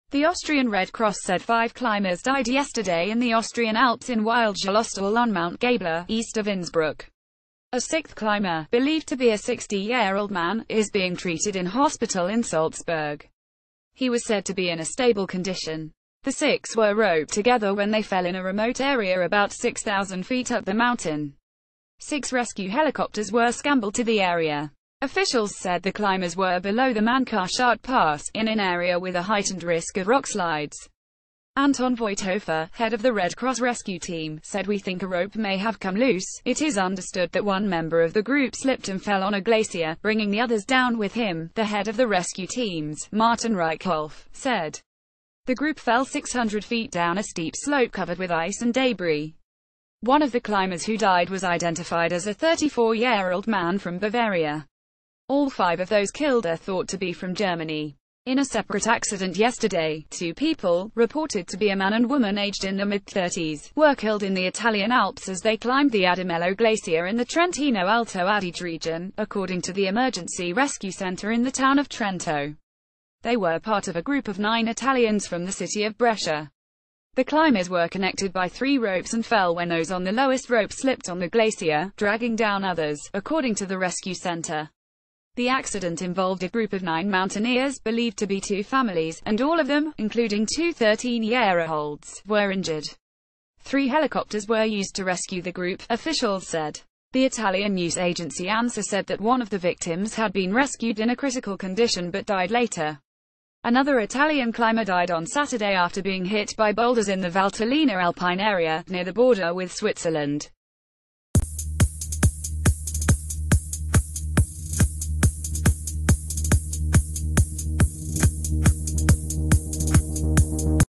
[News]Five climbers roped together fall 600ft to their deaths in alps
Merci de regarder.
N'oubliez pas aimer, commenter, partager et souscrire sur mon chanel.